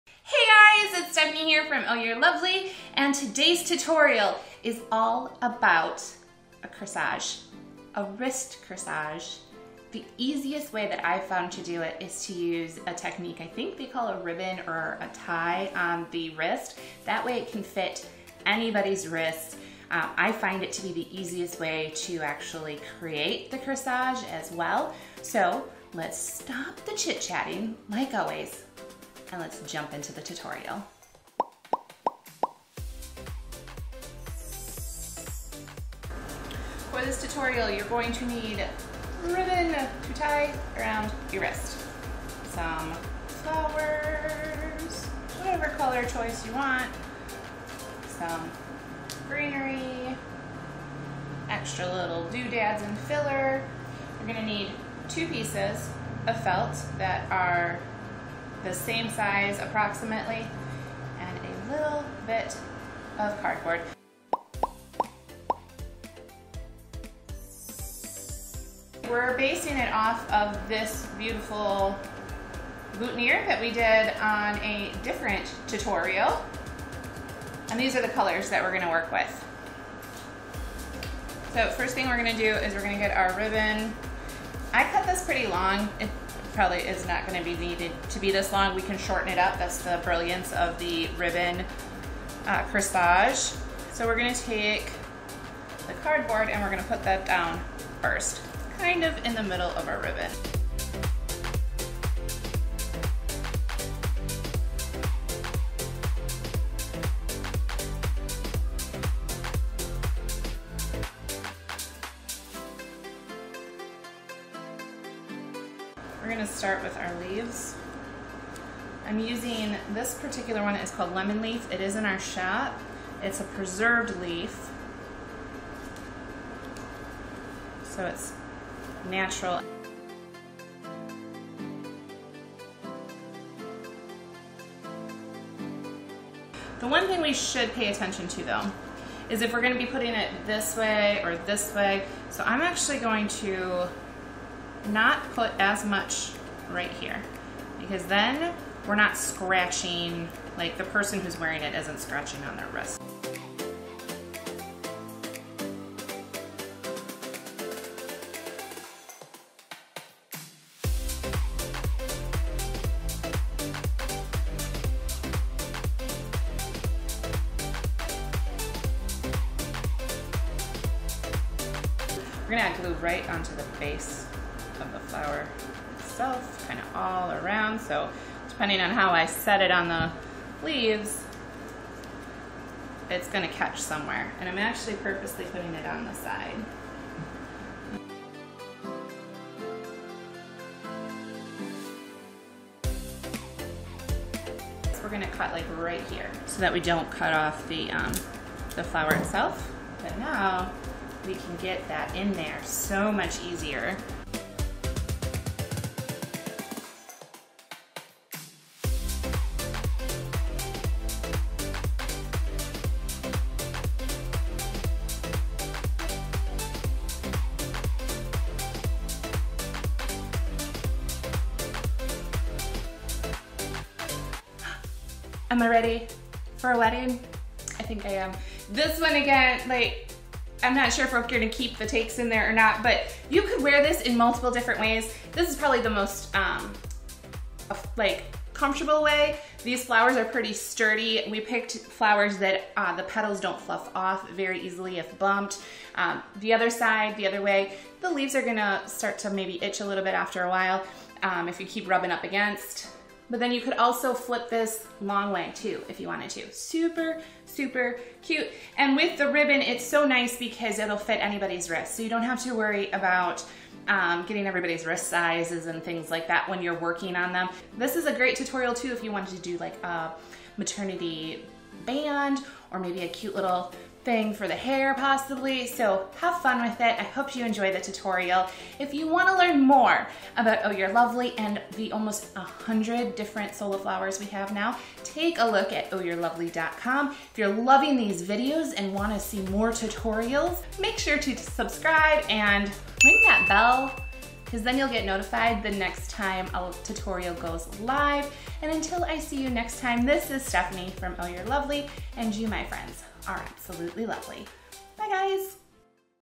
How to make a wrist corsage using Sola Wood Flowers
Are you ready for a wedding?

Create beautiful wedding day corsages using sola wood flowers! A step-by-step guide on how to DIY the perfect wrist corsage for the bride, bridesmaids, and mother of the bride.

Here are the materials you'll need to get started:
ribbon
sola wood flowers
greenery
2 pieces of felt (same size)
cardboard
glue gun